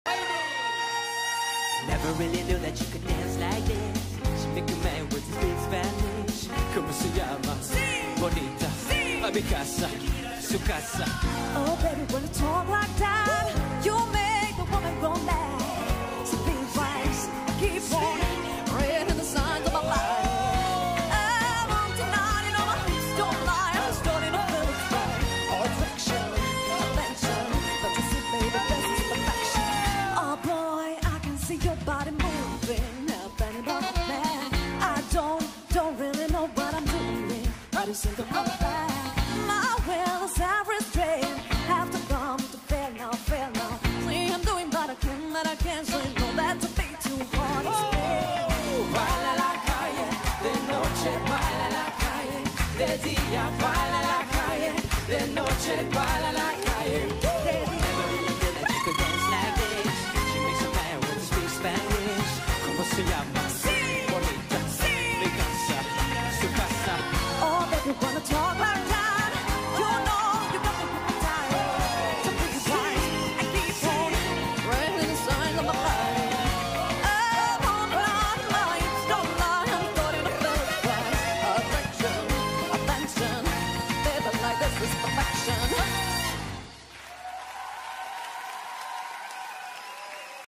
Gaby Spanic Volverá a Hungría Para Ser Parte Del Jurado en Dancing With The Stars
Gaby Spanic
Gabriela Spanic
Paola Bracho
A Usurpadora
La Usurpadora
La Intrusa
Paula es Paulina